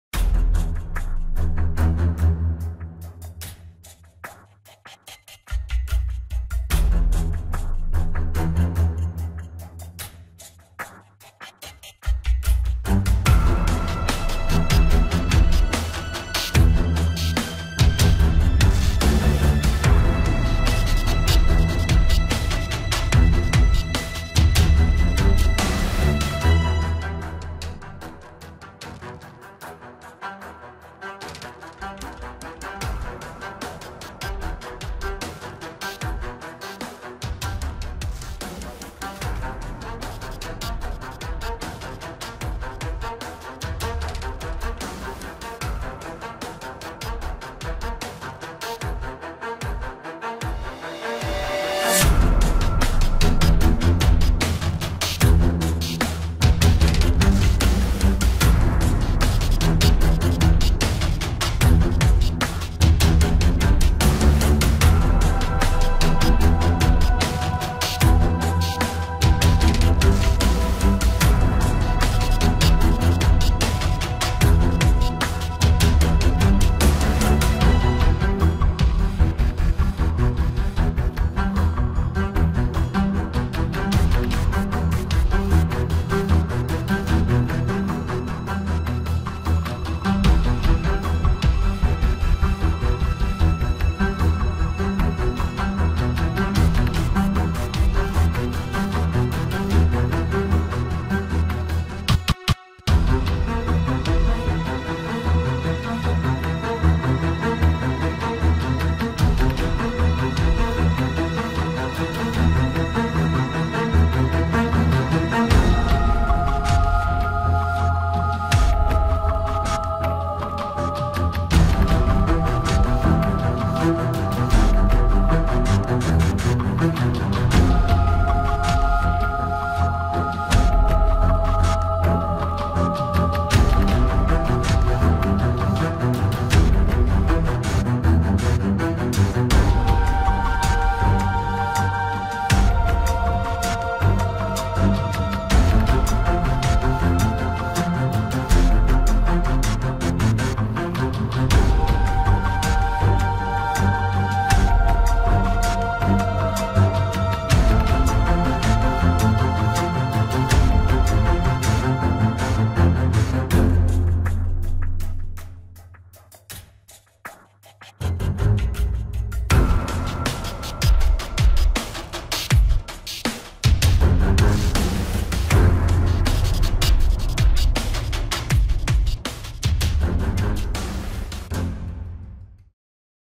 Terrible Power Wide - Komodo Dragon Attack Crocodile
OMG - Close-up of Komodo Dragon attacking crocodile in his lair.